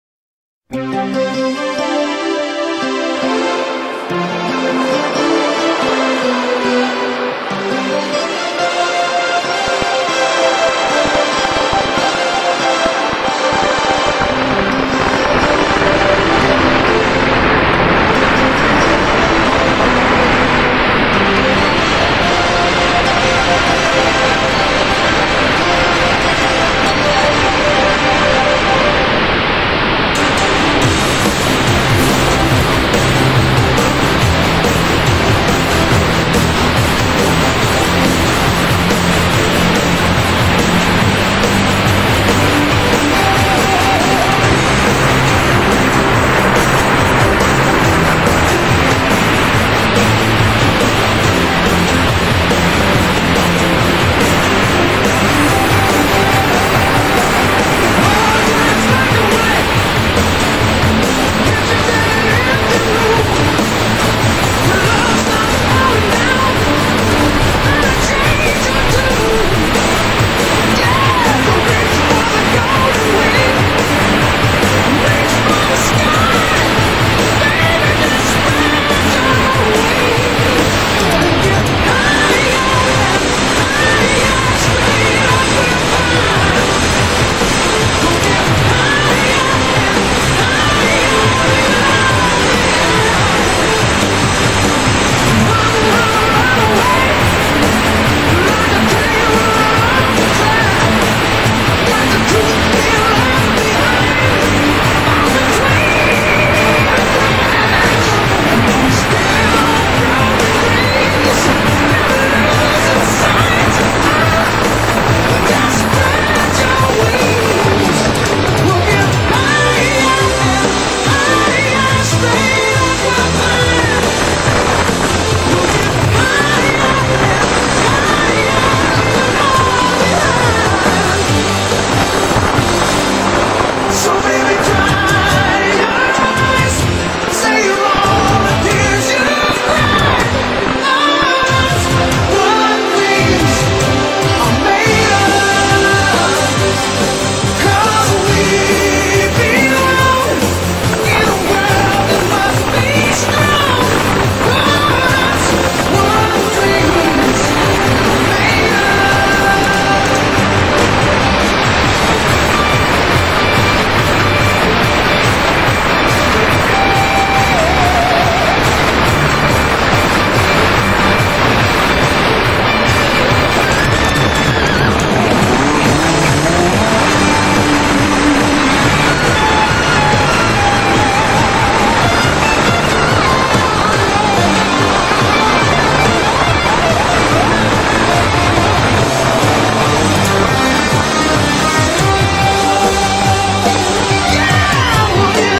F/A-18C Hornet Blue Angel 90mm With Effects
Thought I would explore Windows 10 Photos software which comes with its own video editor.  Got some cool 3D effects...